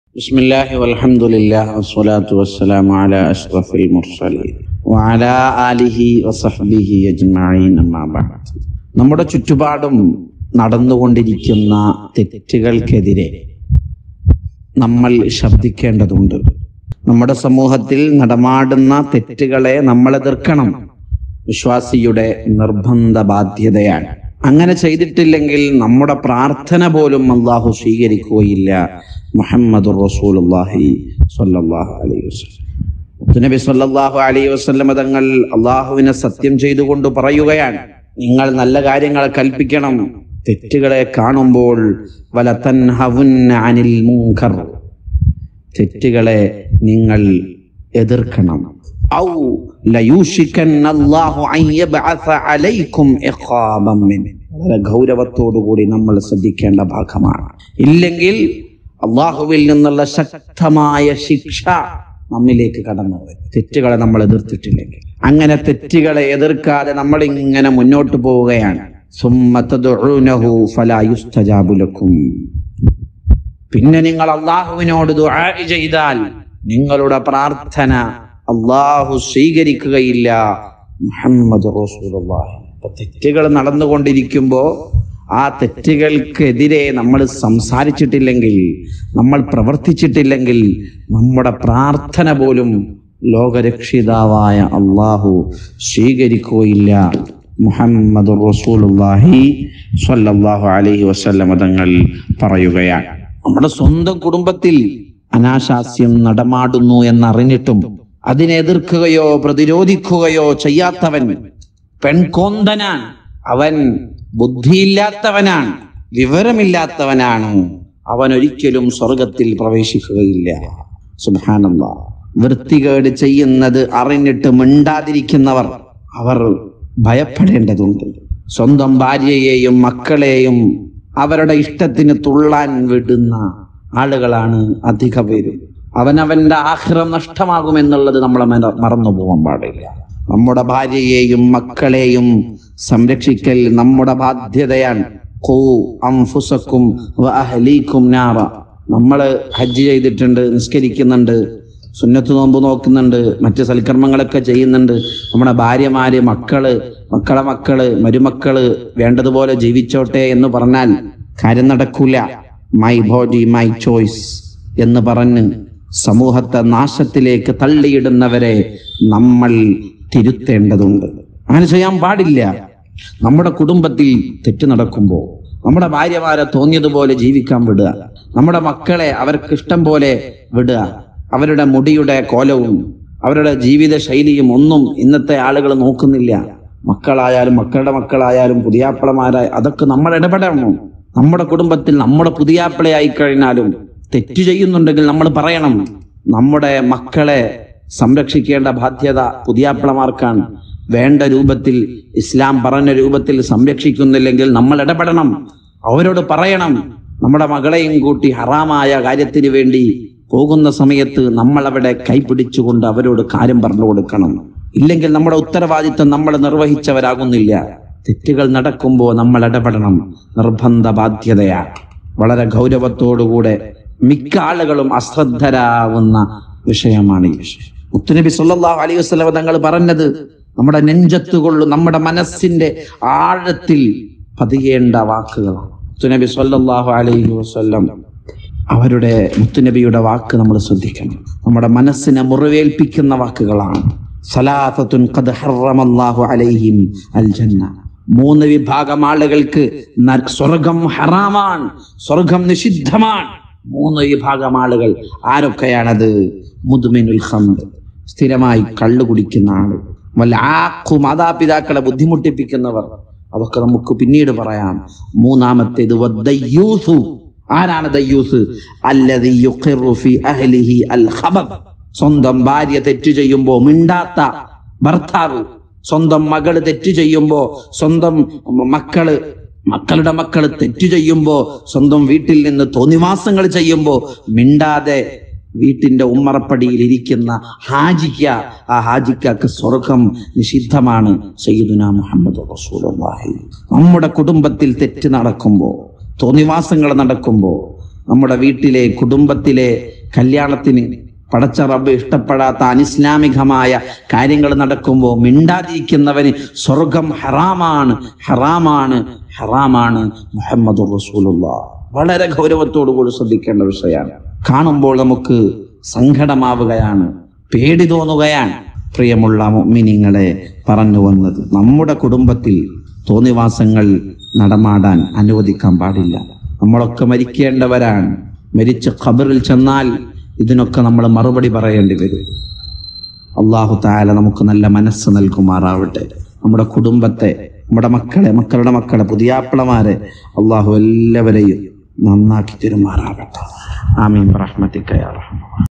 ഇങ്ങനെയുള്ള കൂട്ടർ നിങ്ങളെ കുടുംബത്തിൽ ഉണ്ടോ?സ്വർഗ്ഗം ഹറാമാണെന്ന് മുത്ത് നബി|Dr.MohammedSHAFI AZHARI
KMIC Live Islamic Channel ലോകത്തിന്റെ വിവിധ ഭാഗങ്ങളിൽ നിന്നുള്ള തത്സമയം പ്രോഗ്രാമുകൾ  വിരൽ തുമ്പിൽ ലഭിക്കാൻ ...